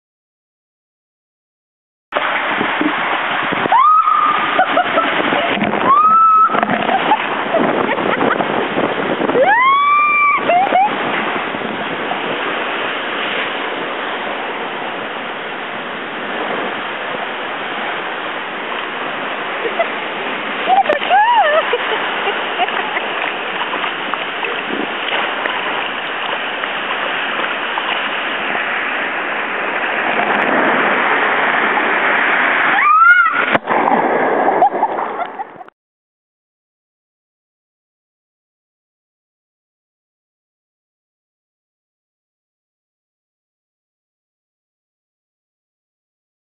Dragon's Den, Take 1 - Splish Splash Water Park
One of most cool ride on the park... sometimes You I'll get stucked on the way out and somebody will bump on you.. But that is the fun of it!!!